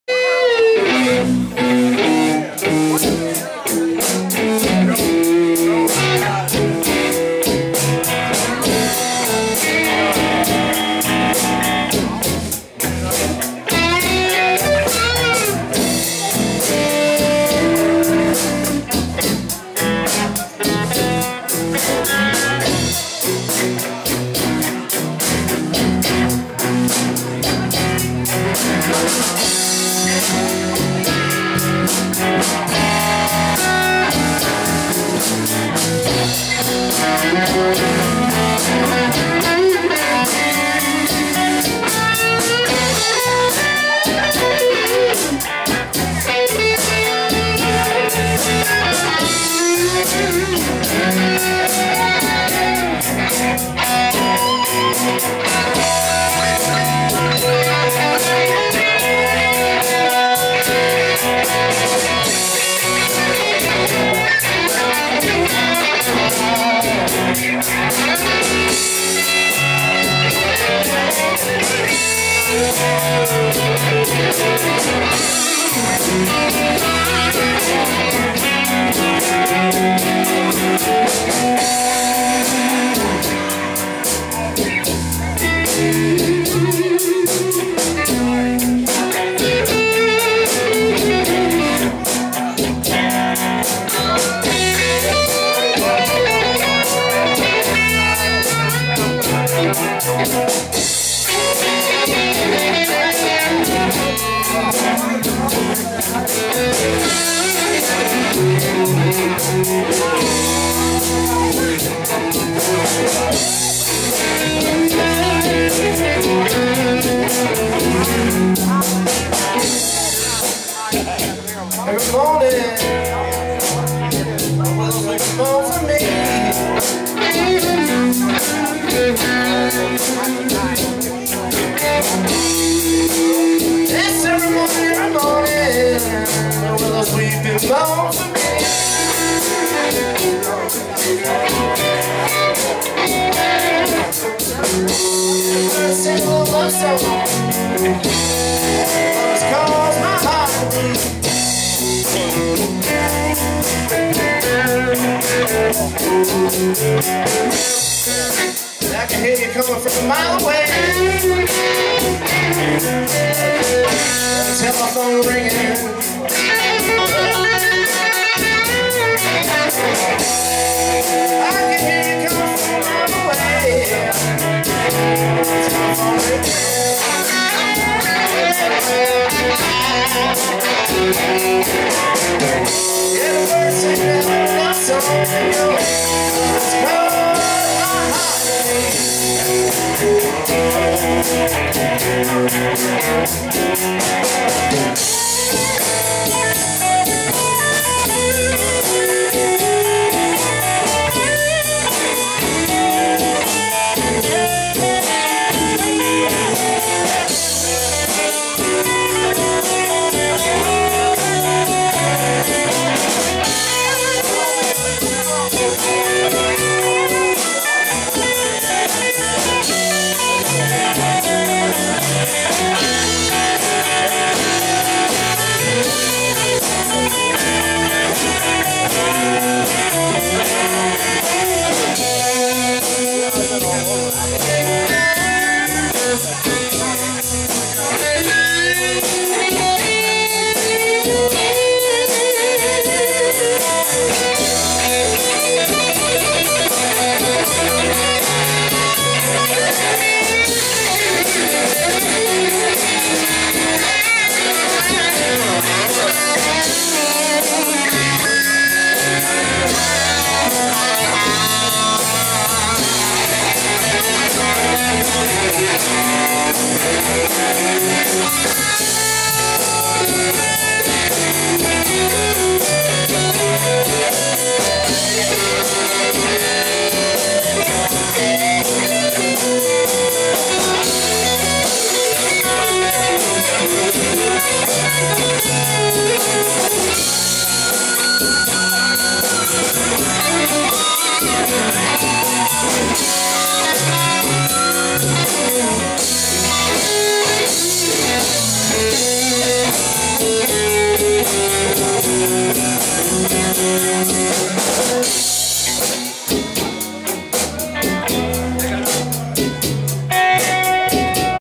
HIRAM STATION...3.26.13...#8.....ROBERT HEARNDON
HIRAM STATION...3.26.13...#8. ROBERT HEARNDON VOCALS/GUITAR...MIKE CABE/BASS. JEFF CALICO/DRUMS. JASON DIEHL/GUITAR...